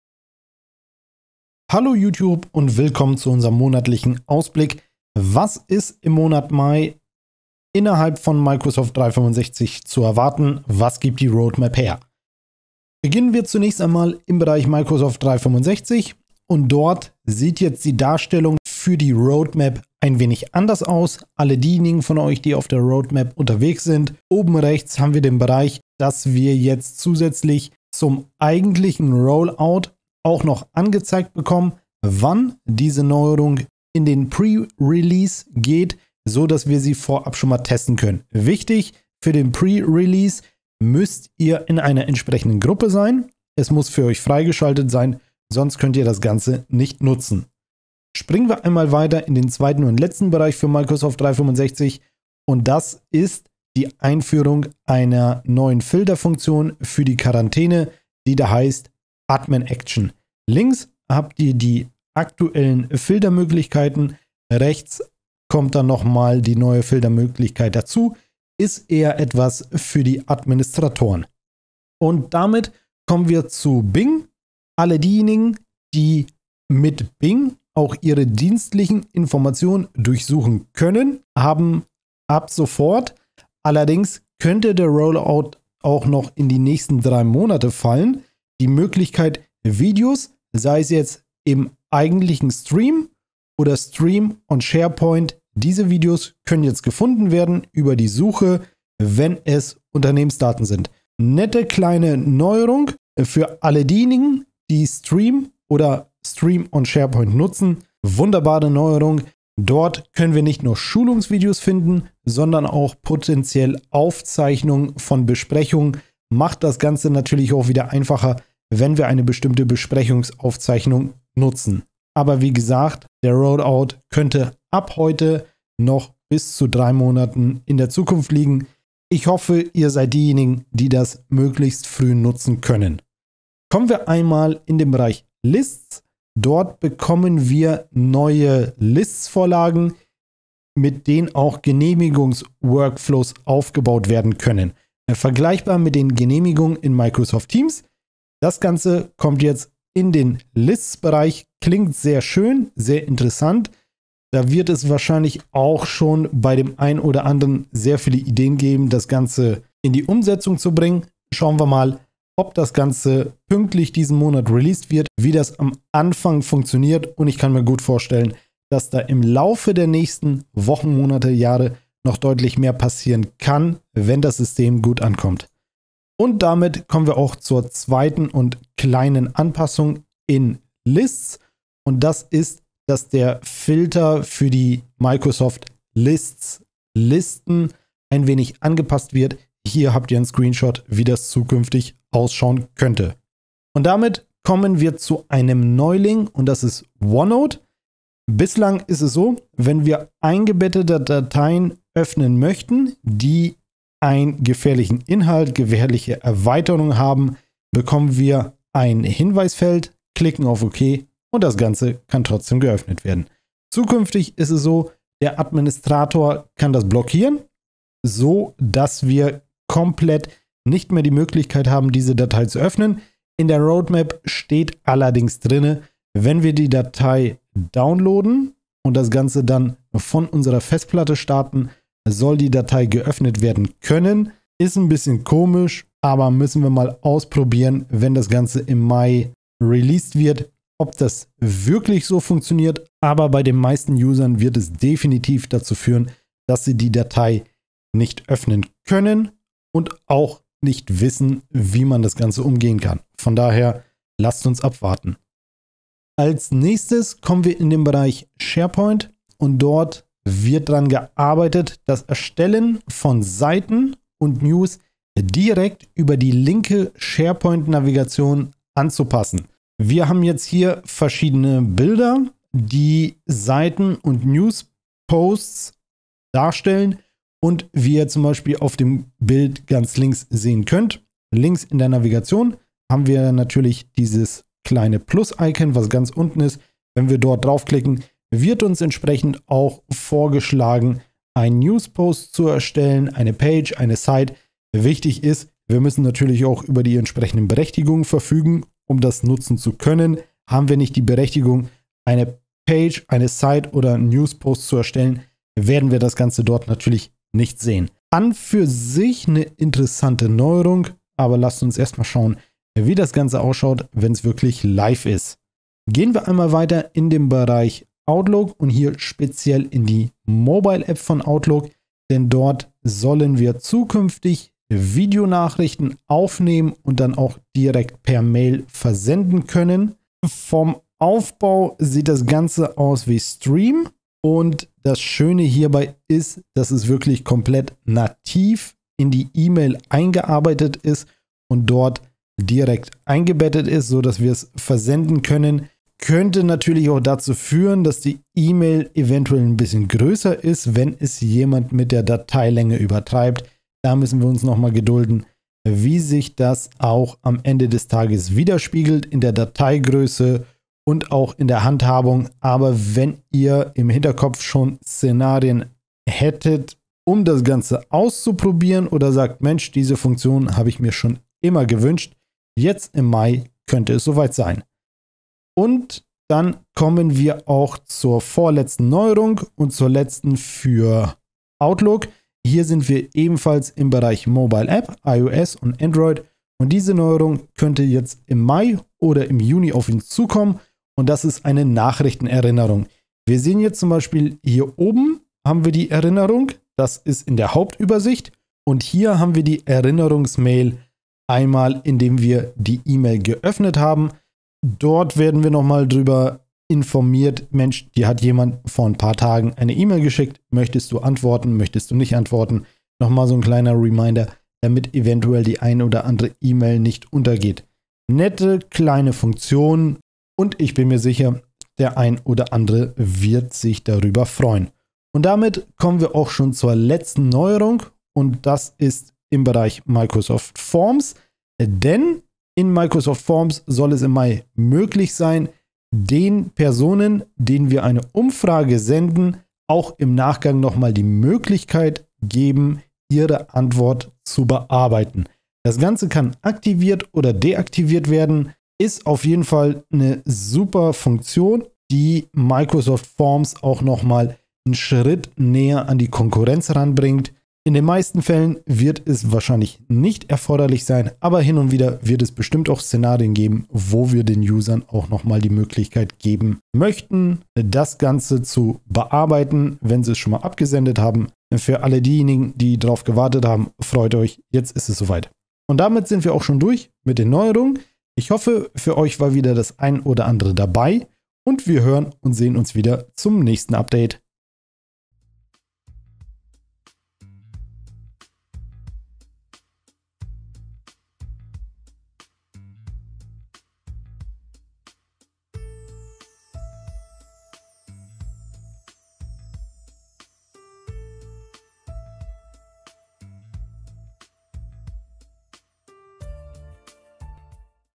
Was ist im Mai für Microsoft 365 zu erwarten? | Mai 2023 Roadmap
Was können wir im Mai 2023 für Microsoft 365 alles erwarten? Wir haben uns einmal die Microsoft 365 Roadmap angeschaut und mehr als 7 Bereiche mit etlichen Neuerungen herausgesucht, die euch gefallen könnten. Im April und darüber hinaus erwartet uns wesentlich mehr, aber leider ist nicht alles davon vorzeigbar oder der Rollout geht über mehrere Monate.

▬▬▬▬▬▬ Abschnitte 🕓 ▬▬▬▬▬▬
0:00 Microsoft 365 Roadmap Mai 2023
0:12 Microsoft365
1:11 Bing
2:13 Lists
3:13 OneNote
4:16 SharePoint
5:16 Outlook
7:05 Forms
7:55 Verabschiedung
8:07 Abspann